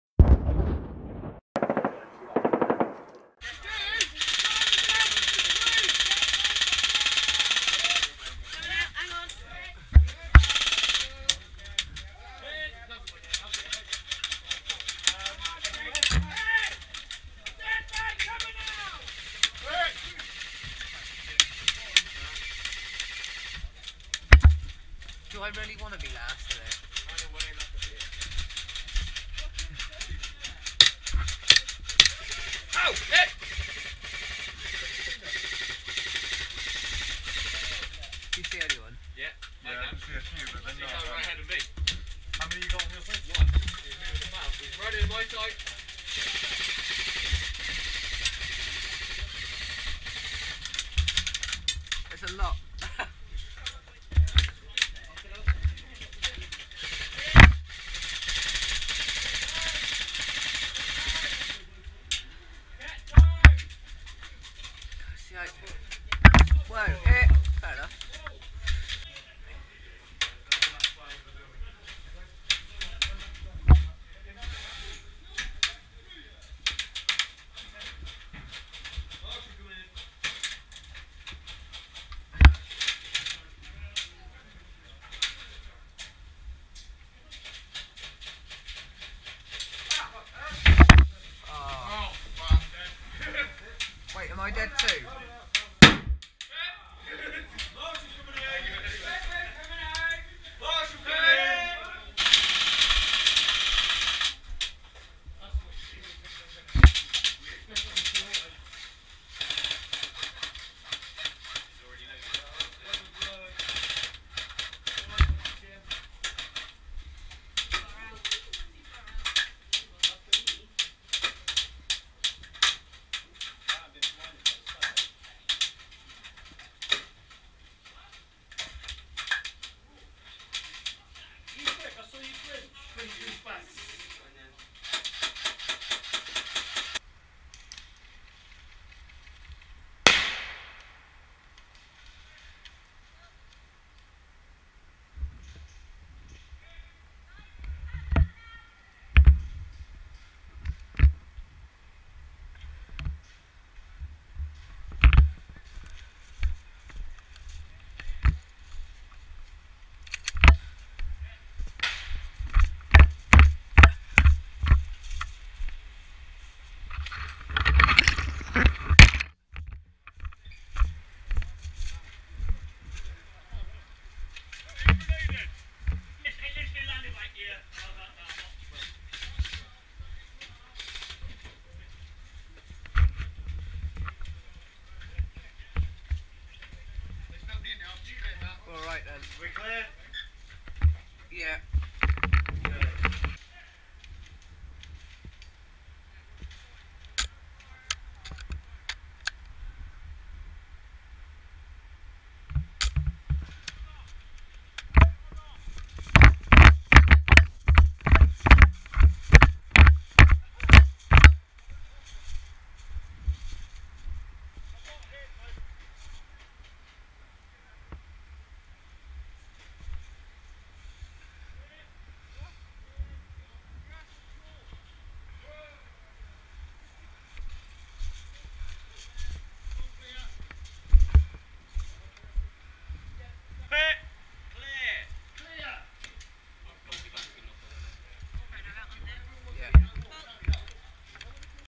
Attacking/defending the buildings! | Airsoft
Another sniping video, this time defending and then attacking a series of 3 buildings.
Thumbnail photography credits to- Clark Evans Photography